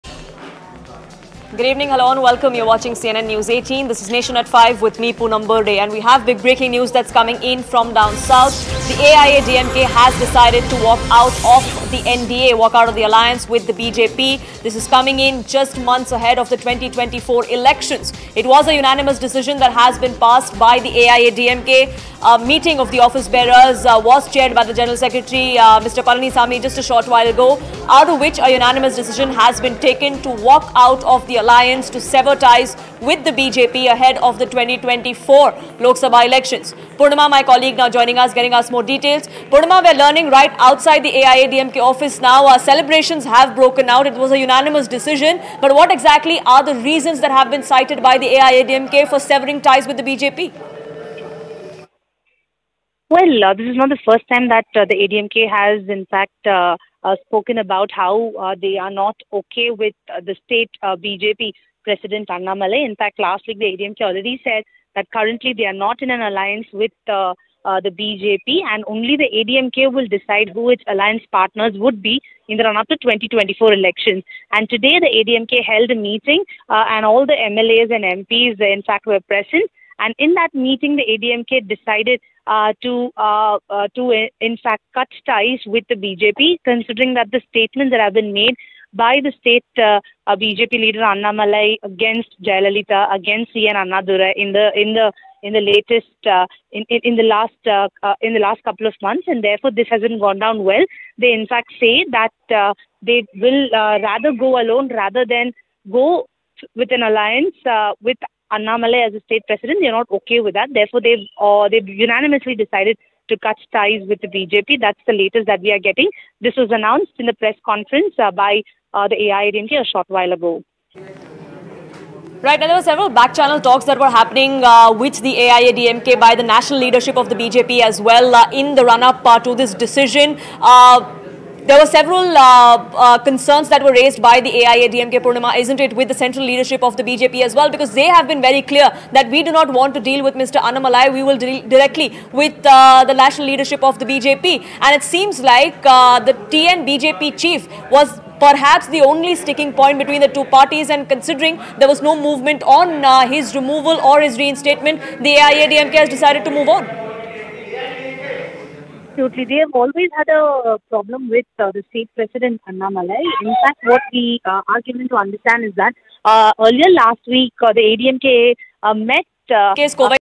NDA Alliance | AIADMK Walks Out Of The Alliance With BJP Ahead Of The Lok Sabha Elections | News18
AIADMK says it will lead a separate front for the 2024 Lok Sabha polls. AIADMK General Secretary Edappadi K Palaniswami chaired a party meet of key office-bearers, including the district secretaries and MLAs on Monday. In the meet, AIADMK passed a resolution to break off from the Bhartiya Janata Party (BJP)-led National Democratic Alliance (NDA).

n18oc_breaking-news